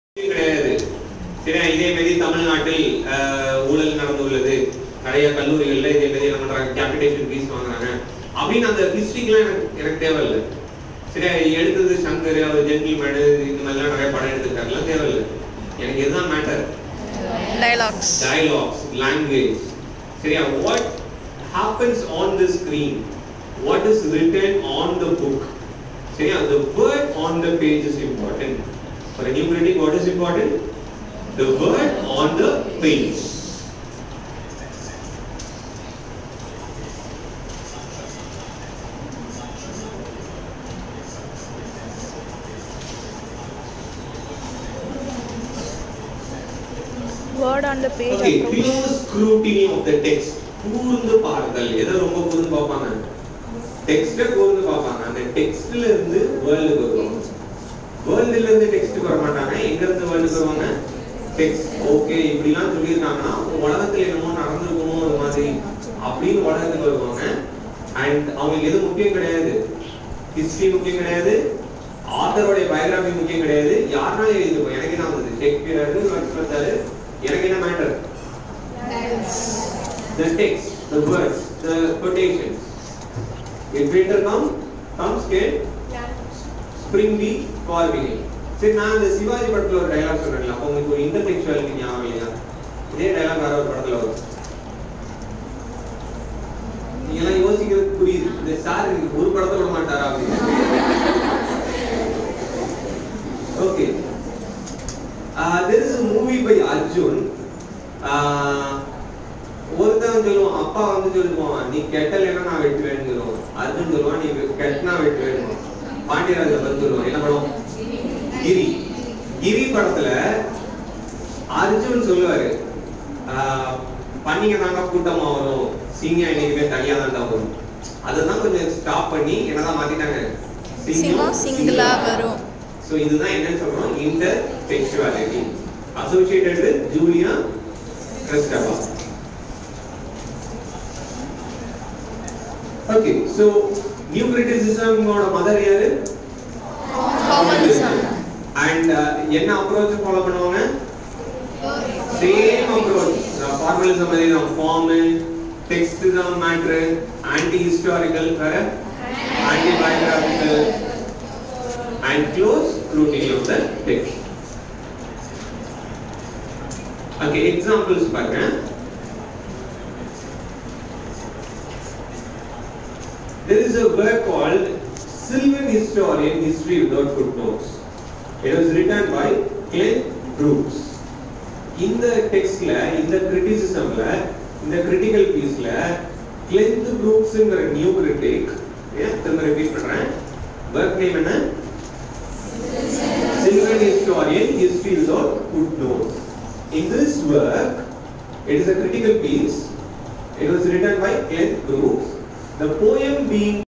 Formalism and New Criticism 7
Formalism and New Criticism by K. Siva Rama Krishnan, Assistant Professor of English, Rani Anna government Arts College for Women, Tirunelveli.